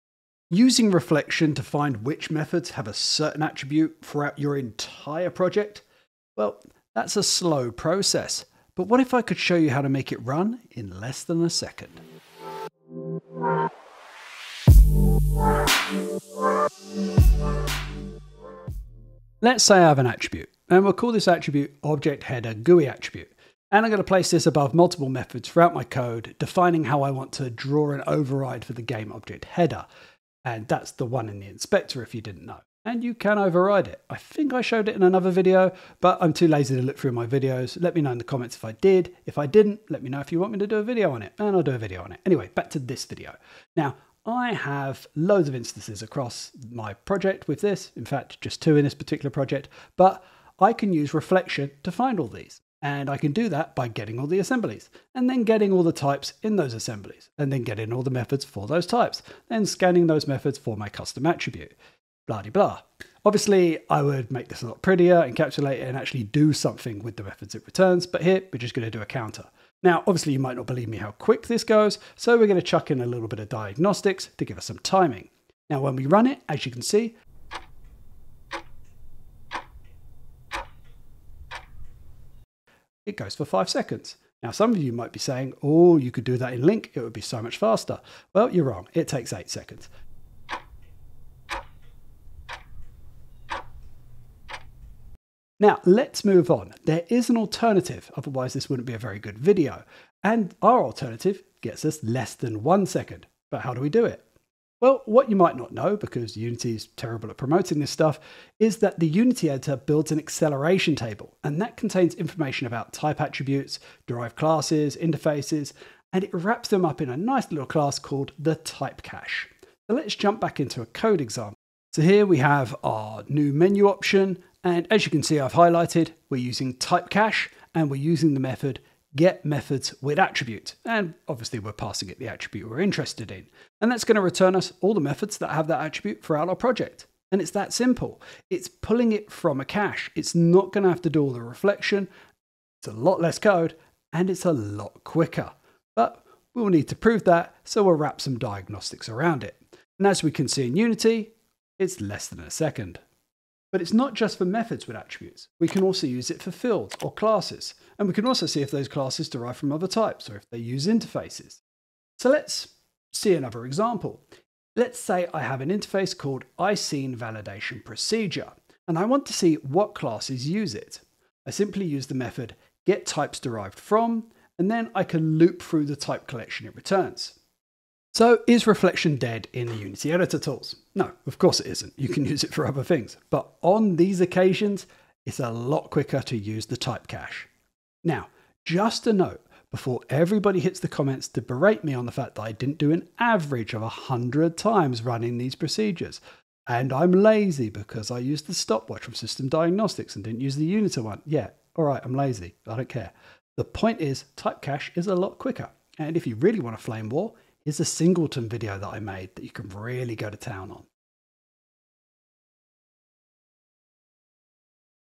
Is Reflection Dead in Unity?
Using reflection to find which methods have a certain attribute throughout your entire Unity project is a slow process, but what if I could show you how to make it run in less than a second.

☞ Asset Links

🛈 *Documentation Links*